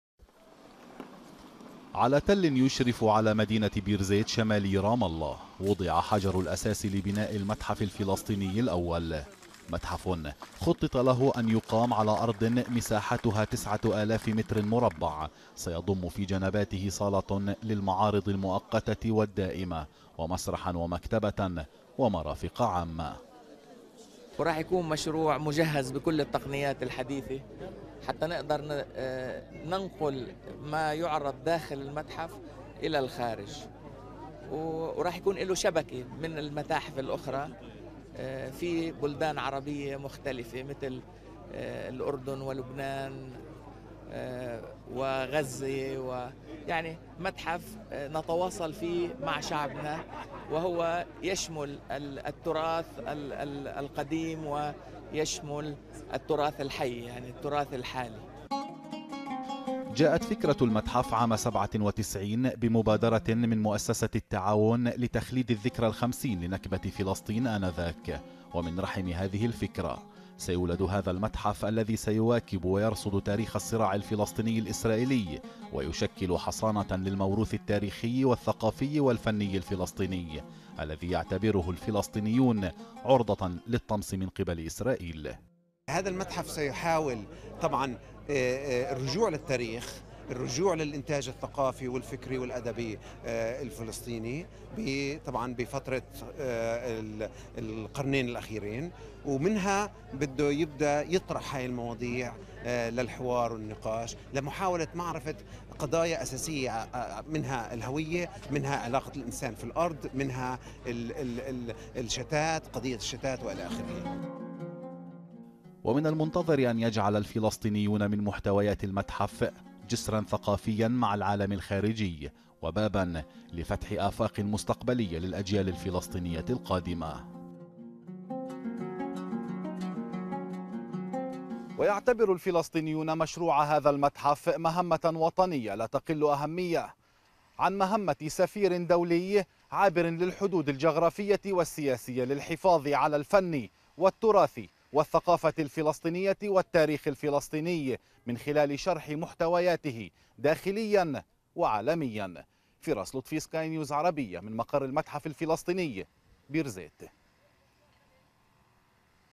وضع حجر الأساس لمتحف في رام الله
تقرير قناة سكاي نيوز عن حفل وضع حجر الاساس بتاريخ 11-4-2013